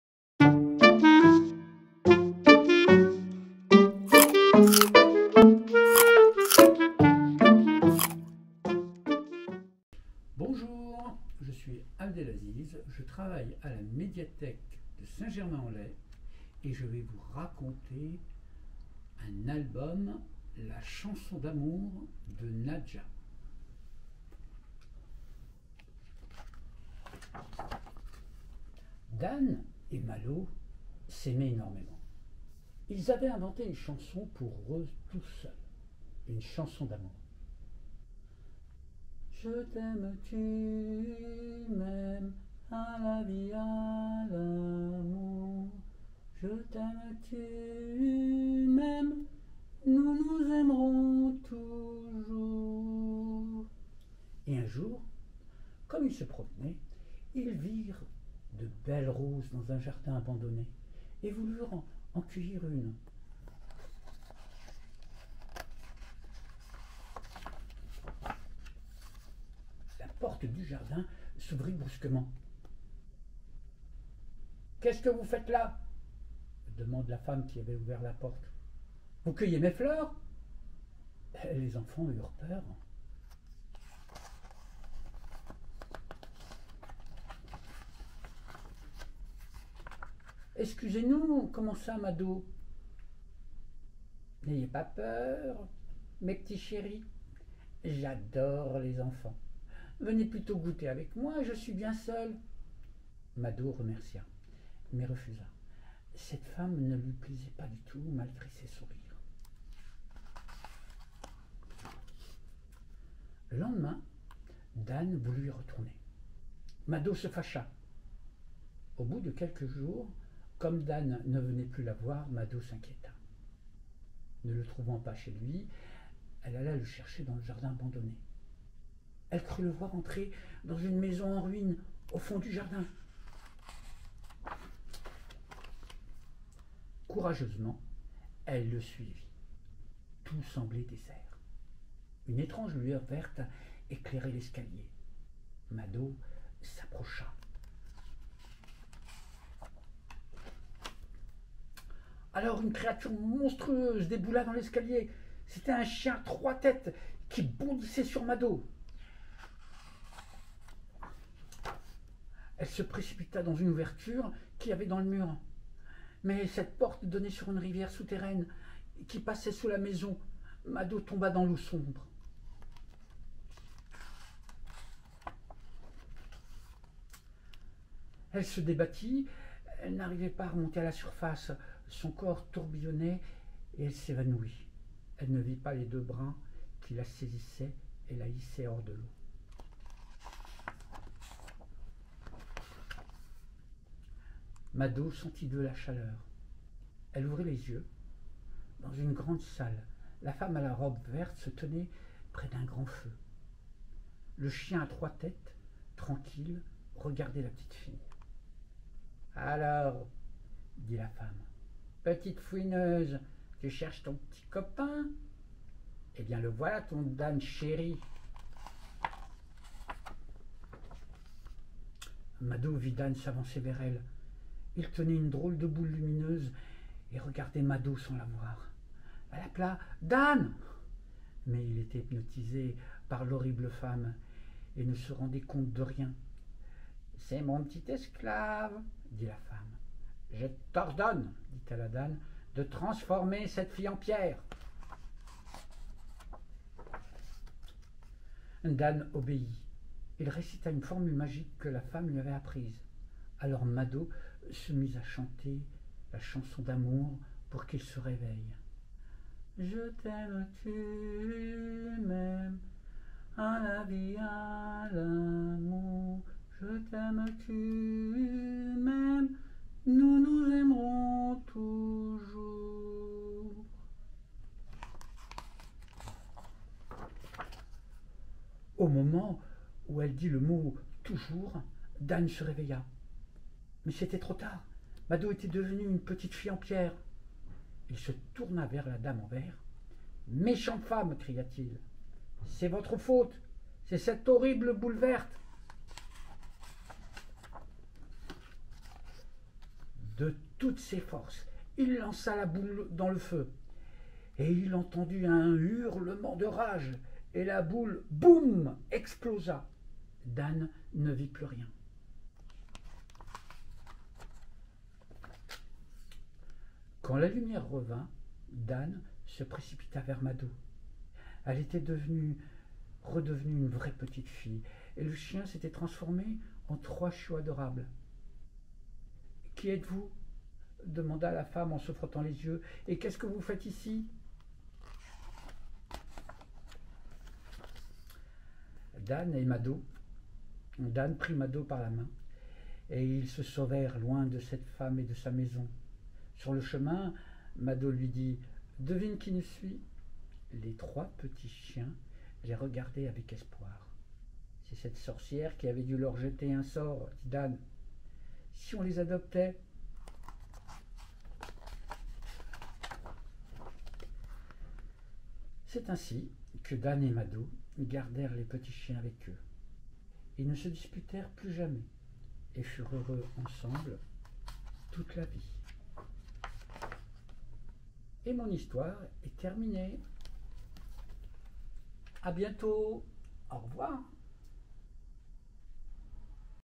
Croqu'histoires : "La chanson d'amour", de Nadja (L'école des loisirs)
Ils ne se quittent pas. Ils s'aiment énormément. Ils ont même inventé une chanson pour eux, une chanson d'amour. Un jour, derrière un vieux mur, ils découvrent un jardin, le jardin d'une magicienne aux pouvoirs maléfiques...